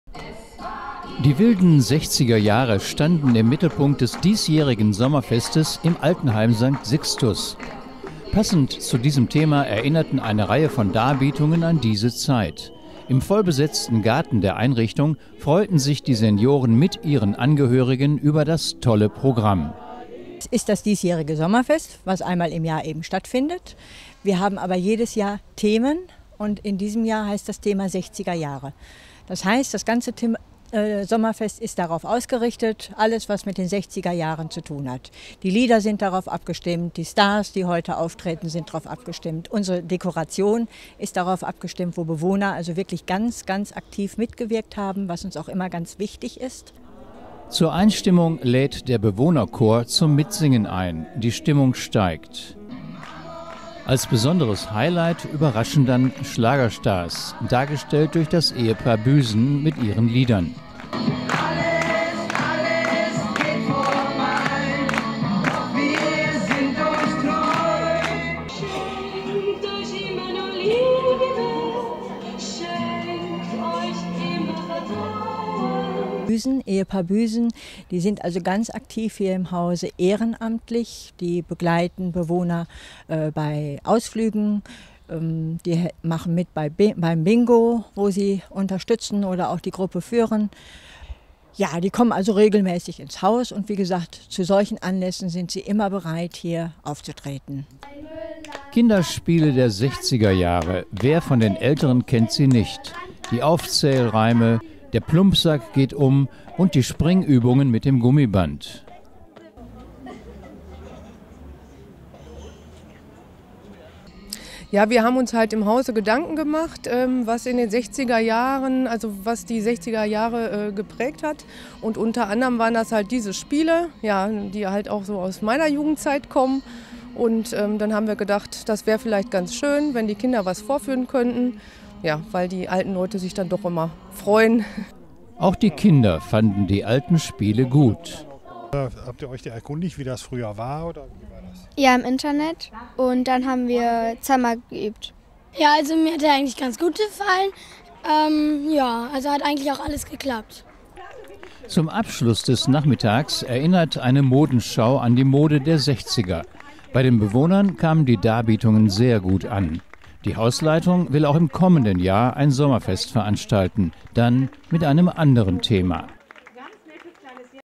Sommerfest im Altenheim St. Sixtus
Die wilden 60er Jahre standen im Mittelpunkt des diesjährigen Sommerfestes im Altenheim St. Sixtus. Passend zu diesem Thema erinnerten eine Reihe von Darbietungen an diese Zeit.